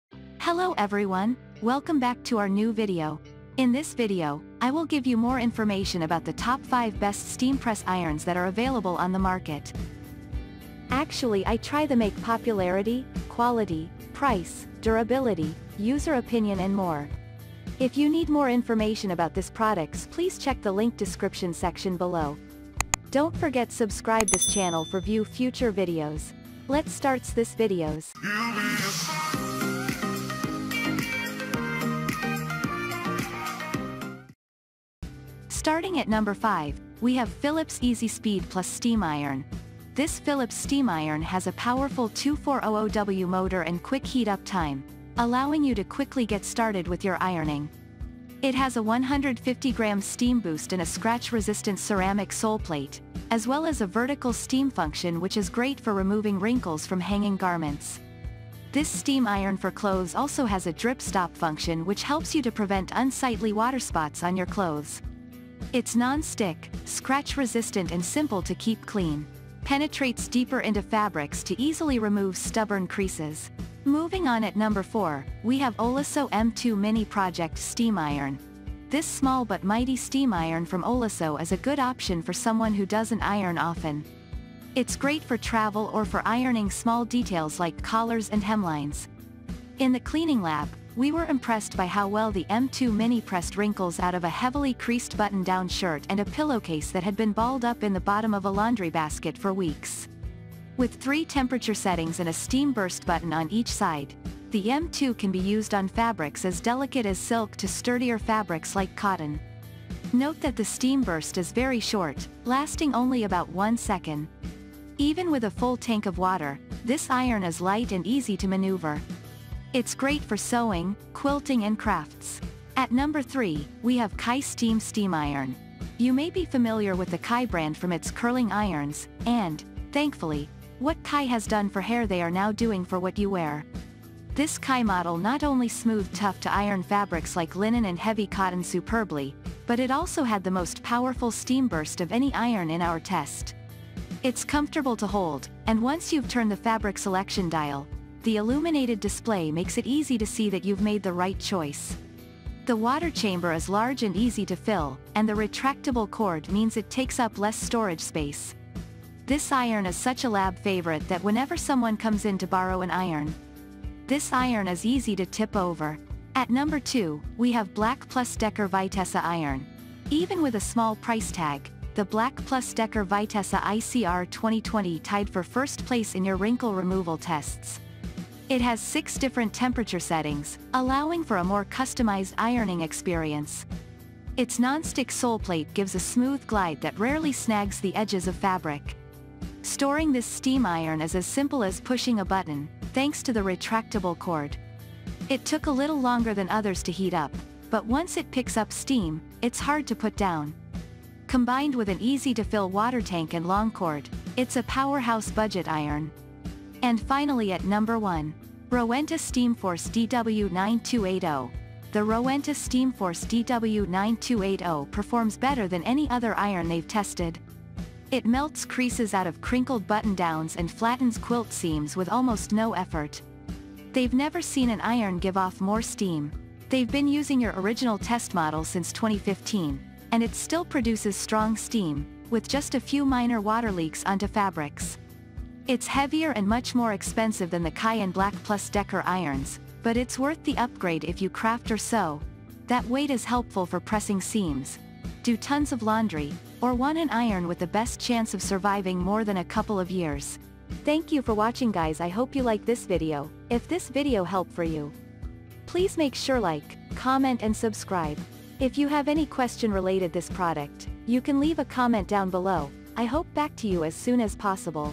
Top 5 Best Steam Press Irons in 2023 reviews
☛ All The Links to Best Steam Press Irons listed in this Video:-

► 5.  Philips Easy Speed Plus Steam Iron

► 4. oliso M2 Mini Project Steam Iron

► 3.  CHI Steam Steam Iron

► 2.  Black + Decker Vitessa Iron

► 1.  Rowenta SteamForce DW9280

In This Video, I listed the Best Steam Press Irons That are available in the market. I made this list based on my personal research, and I tried to list them based on their price, quality,  and many more things.